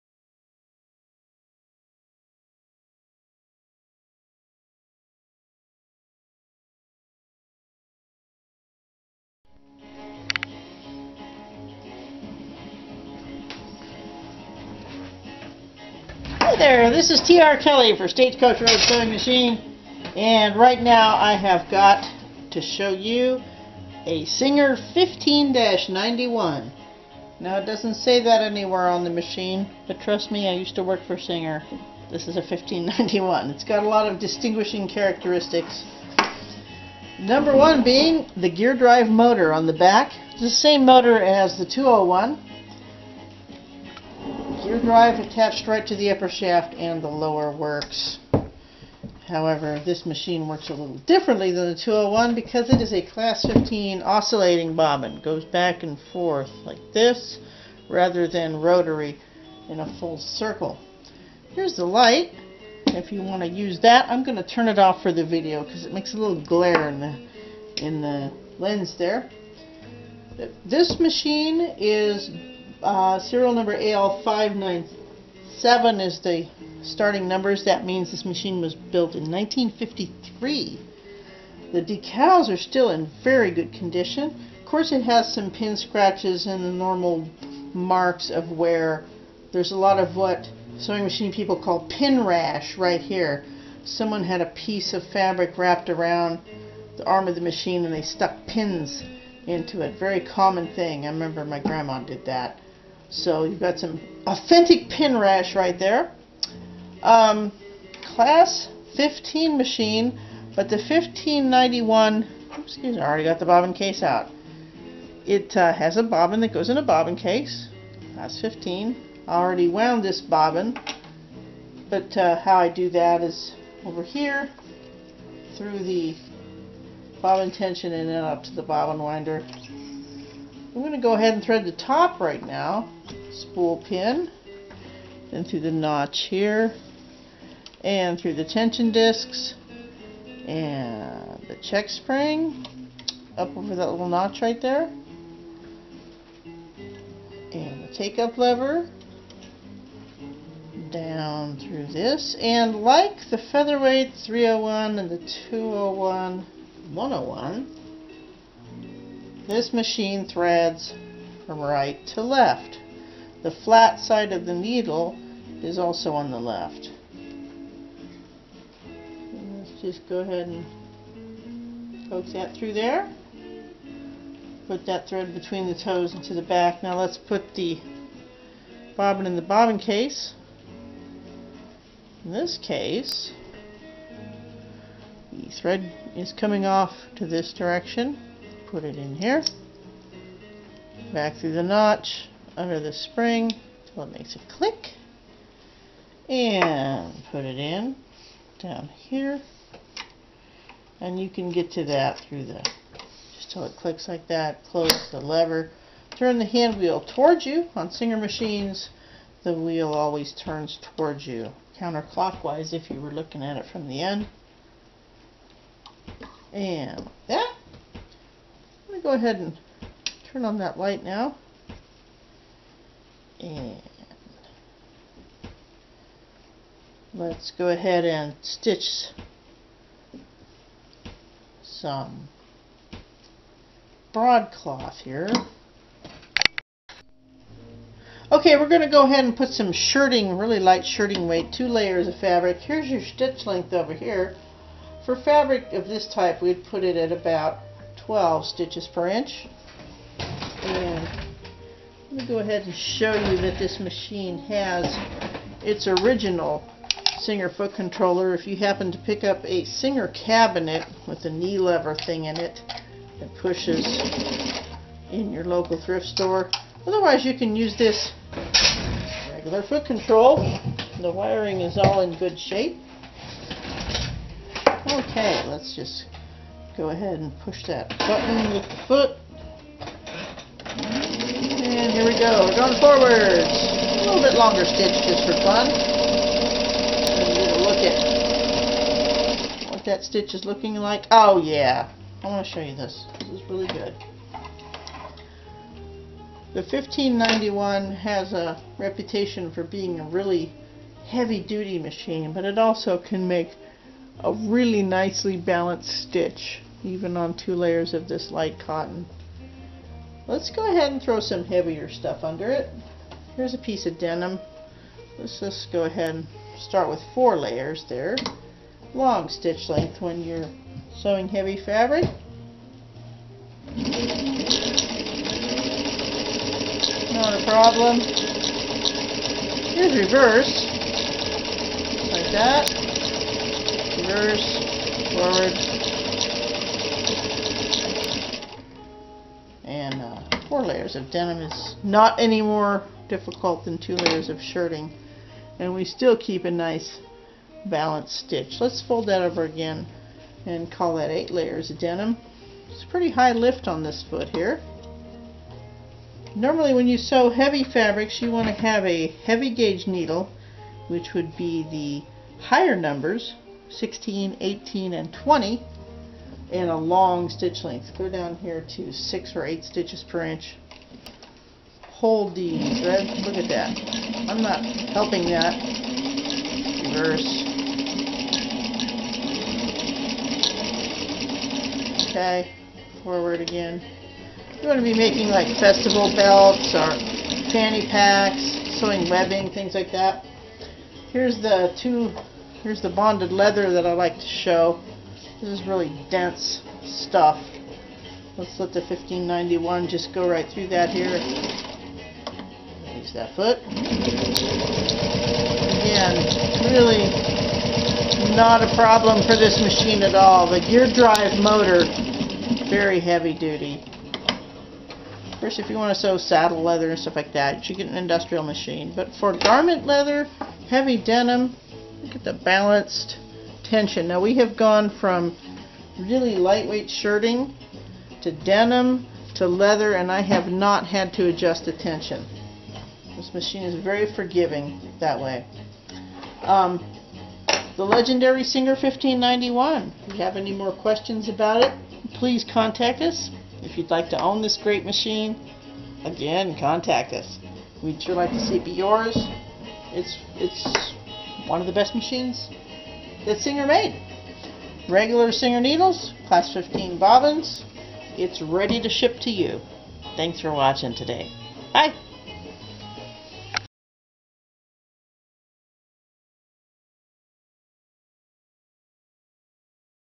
Demonstration Video of a Singer 15-91 Sewing Machine
Watch this video to see how to thread the machine, wind the bobbin, insert the bobbin into the bobbin case and sew on shirting, thick layers of denim, and leather.